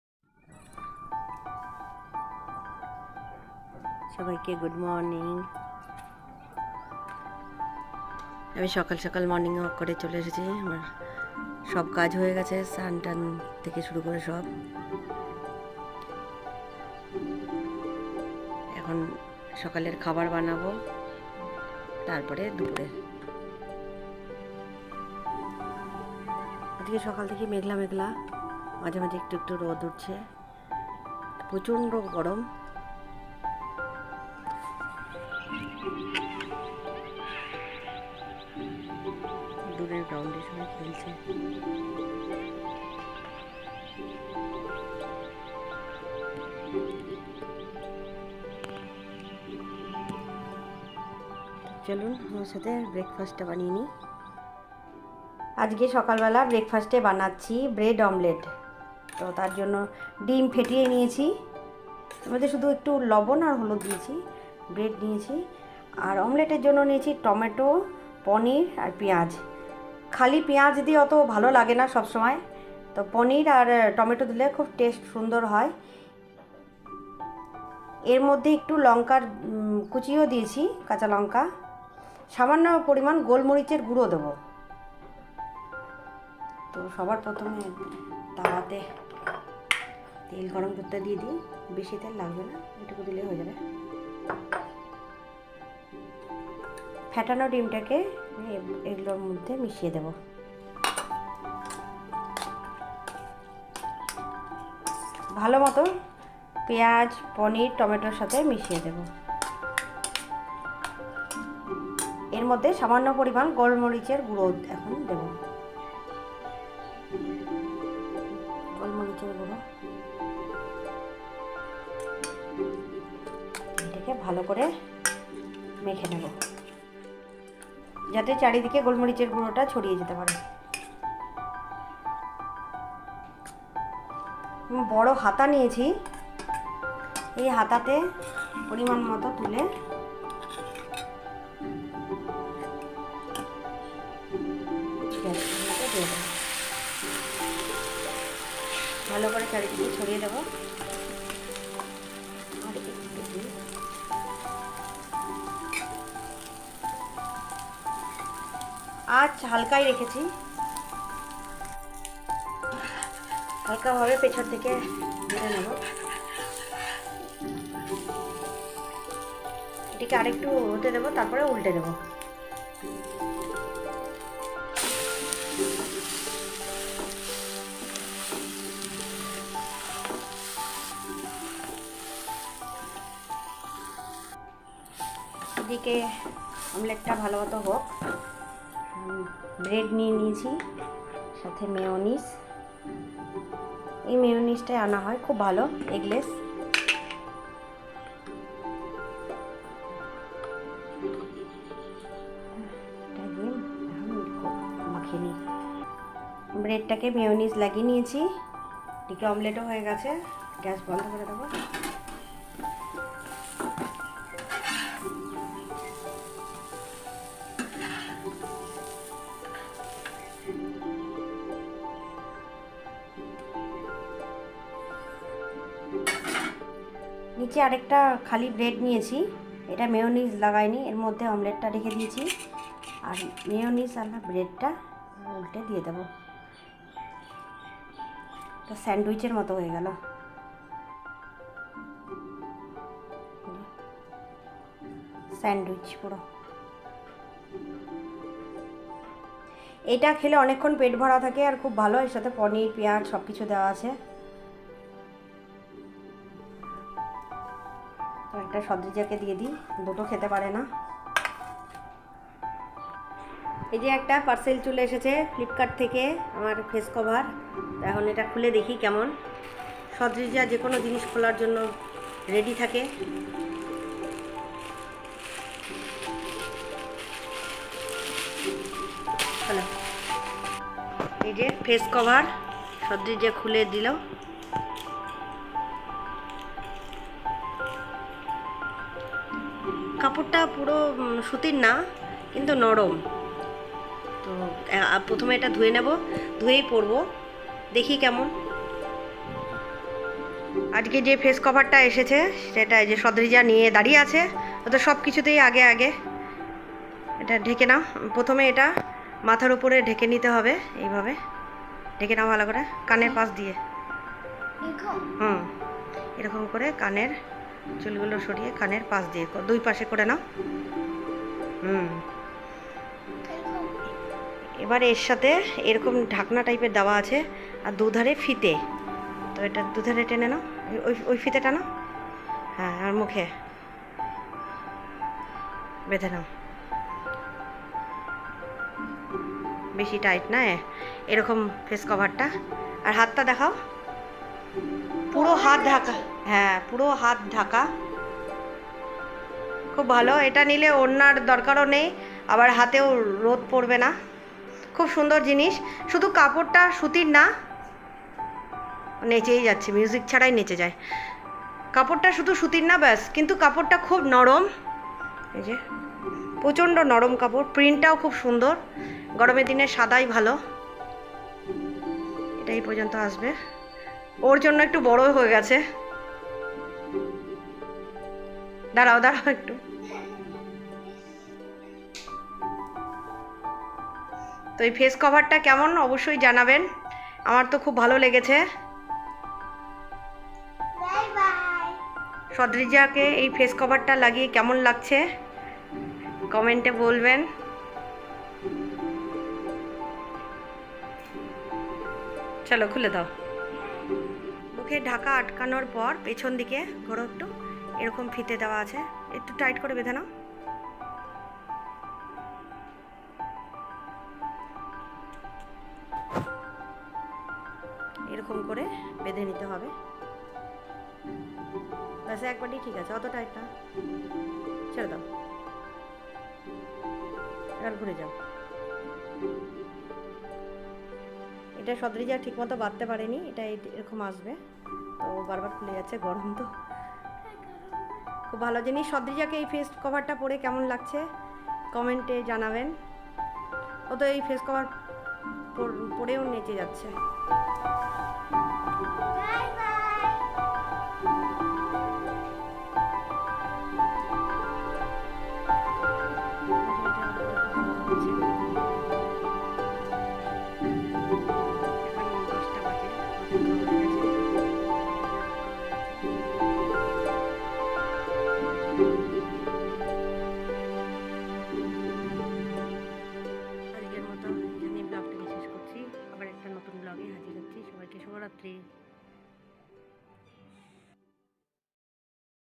চলে এল আমার ফেস কভার#পনির ডিমের ওমলেট দিয়ে স্যান্ডউইচ#simple life in Odisha 🙏
পনির টমেটো পেঁয়াজ লংকা সাথে ডিম আর গোল মরিচের গুঁড়ো মিশিয়ে অমলেট বানাতে হবে ব্রেডে মিউনিস লাগিয়ে তাতে ওমলেট বানিয়ে নিলেই তৈরি পনির ডিমের অমলেট স্যান্ডউইচ